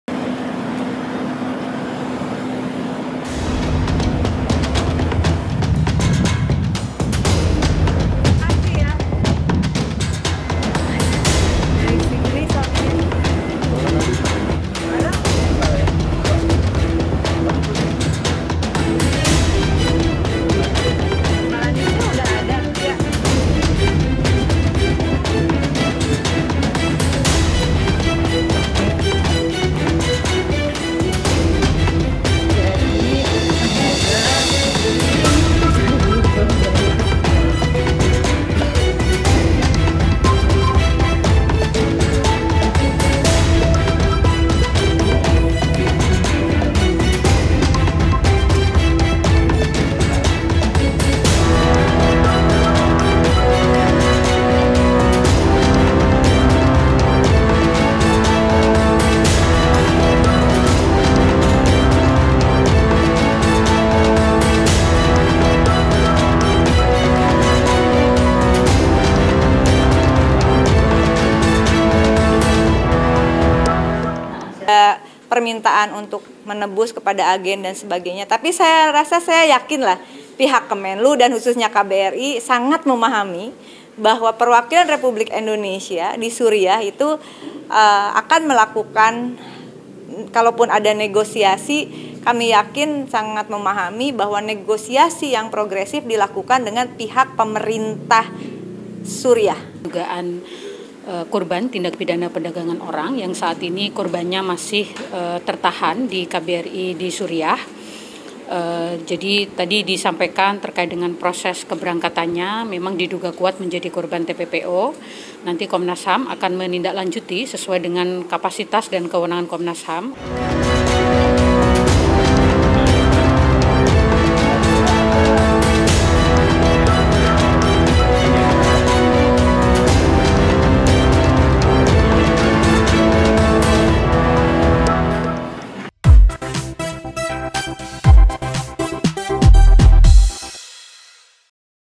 Istri jadi Korban Perdagangan Orang di Suriah, Suami TKW Dede Asiah Ngadu ke Komnas HAM
Suami TKW Dede Asiah, Yongki Hamidun mendatangi Komnas HAM, Jakpus. Yongki didampingi oleh Anggota DPR RI Rieke Diah Pitaloka

Dede Asiah TKW asal Karawang diduga jadi korban perdagangan orang di Suriah. Dede Asiah berangkat sebagai TKI pada tahun 2022 tujuan Turki. Korban dibawa ke Suriah melalui Lebanon dan diduga dijual sebagai budak

TKW itu dikabarkan berhasil diselamatkan oleh pihak KBRI di Suriah. Namun hingga kini, Selasa (1/8) belum ada kabar kepulangan Dede

Kontributor: Rani Stones Sanjaya
Produser: B.Lilia Nova